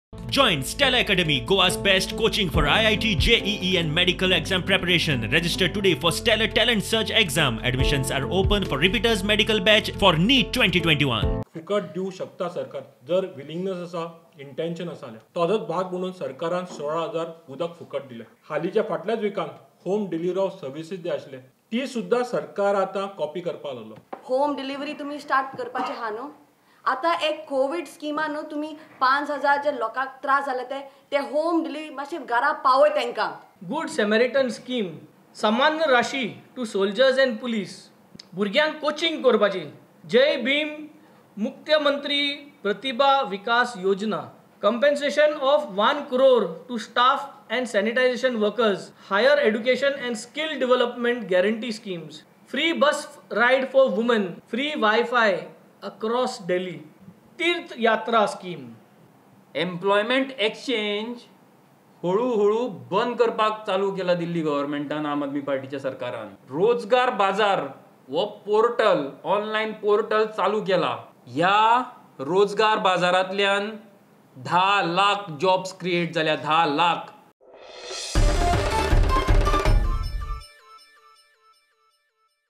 BJP GOVT IN GOA COPYING KEJRIWAL MODEL: RAHUL MHAMBRE
AAP Goa Convener Rahul Mhambre has accused the BJP government in Goa of copying the AAP’s Delhi model of governance.

Speaking to media, Mhambre has said that the BJP government’s fake promises won’t work. For more details watch the video